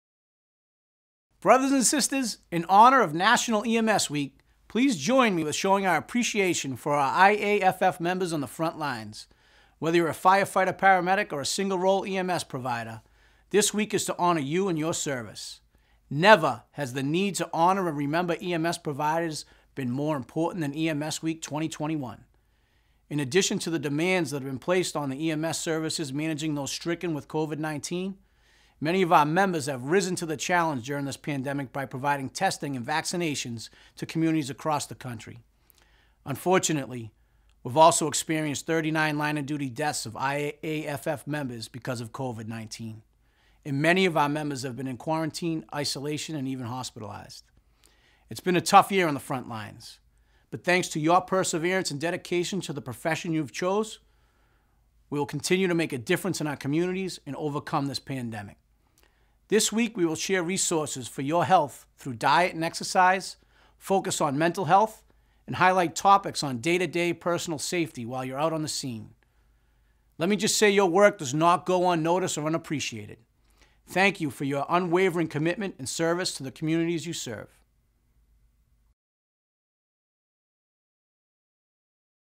EMS Week 2021
IAFF General President Ed Kelly kicks off National EMS Week.